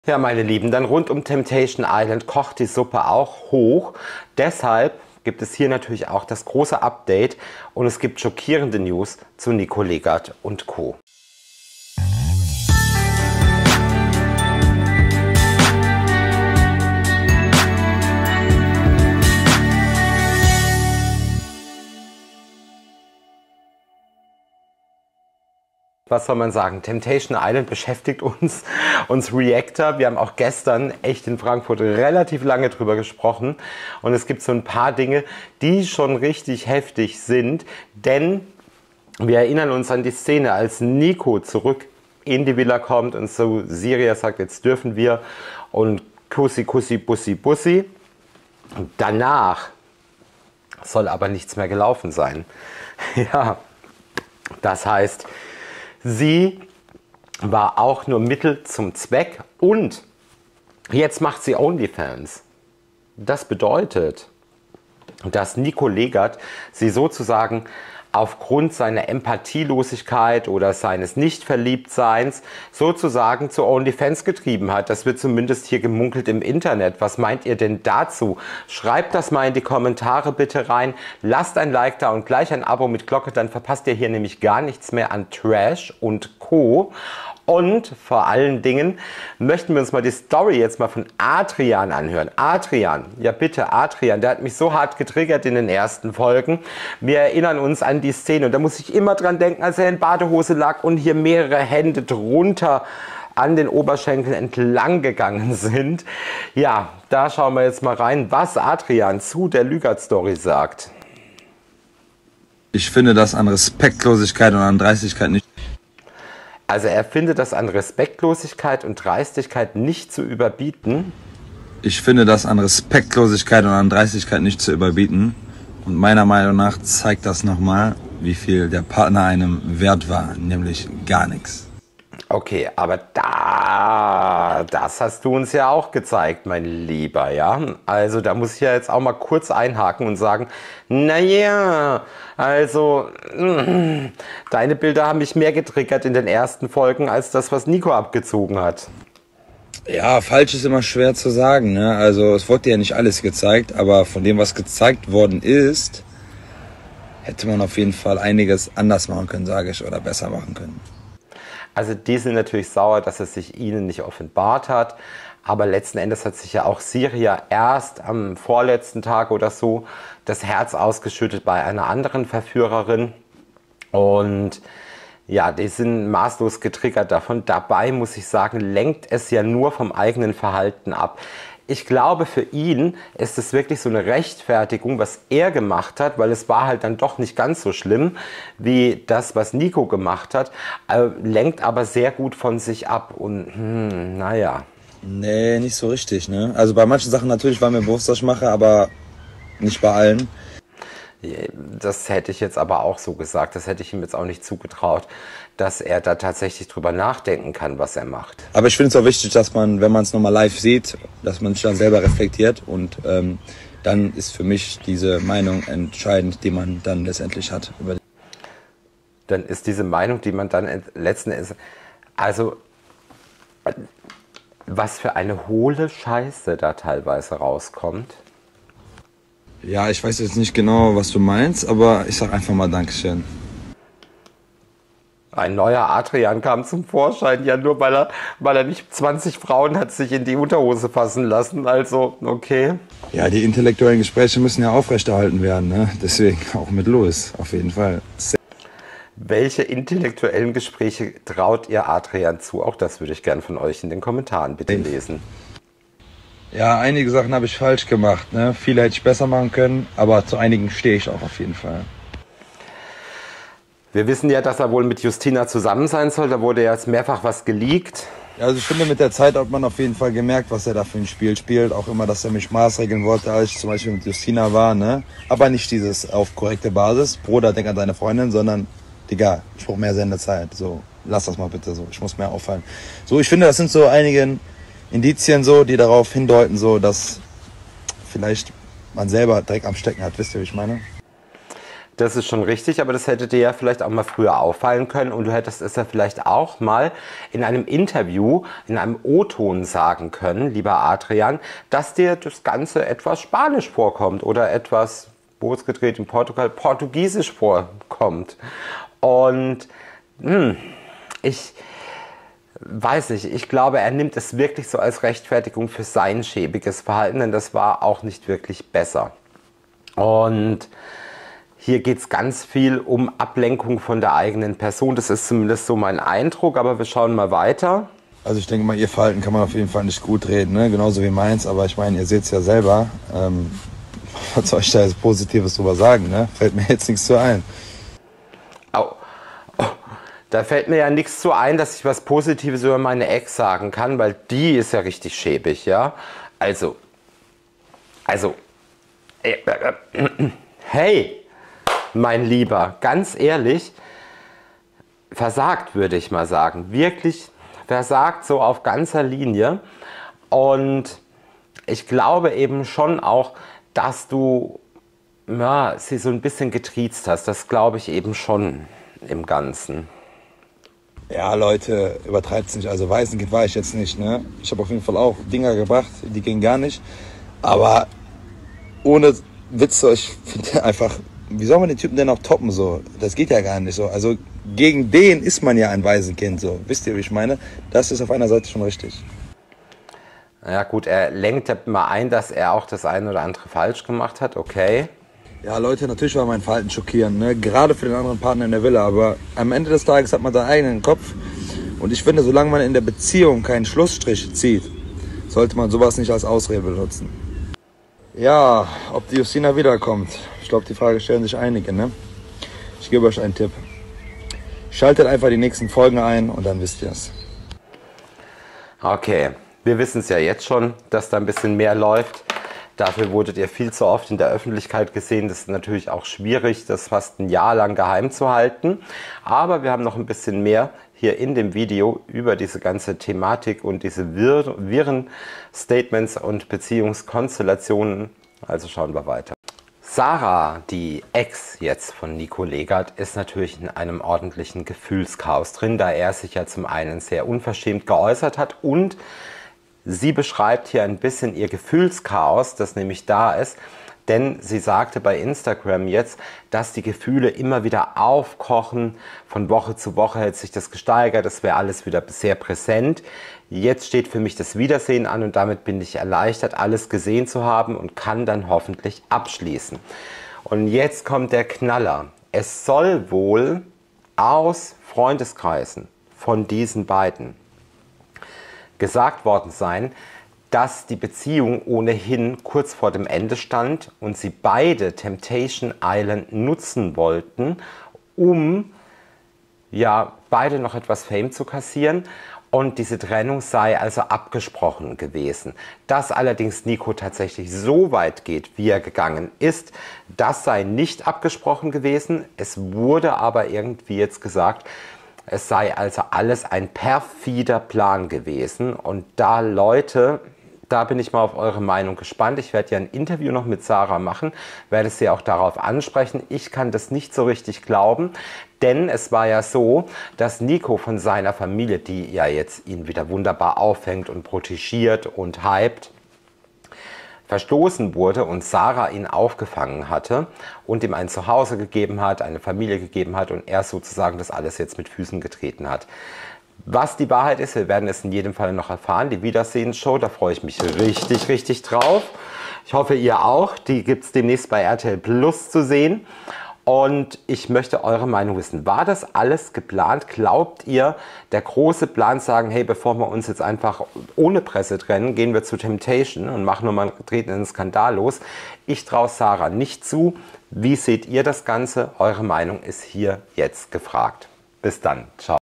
Nico Legat HAMMER: JETZT kommt es RAUS! Der perfide Plan der Trennung! Gossip News!
Dreimal hintereinander ist Nico Legat (25) seiner Freundin Sarah (23) im TV fremdgegangen. Nachdem ihr die Szenen aus dem Badezimmer der Männer-Villa gezeigt wurden, musste sie „Temptation Island“ unter Tränen abbrechen. Noch nie in fünf Staffeln ging der Treue-Test so schief!

Der Sohn von Ex-Fußballer Thorsten Legat (54) hatte sich auf Verführerin Siria Longo (21) eingeschossen – und sich immer wieder unter der Dusche mit ihr vergnügt ...

Hätte er die Fummel-Show nicht vorzeitig verlassen müssen, dann wäre Nico wahrscheinlich noch viel weitergegangen, ist sich Verführerin Nora Lob (33) sicher. „Hätte er die Möglichkeit gehabt, dann hätte er mit jeder Frau dort etwas angefangen. Er fühlt gar nichts, sondern will nur so viele Frauen wie möglich flachlegen!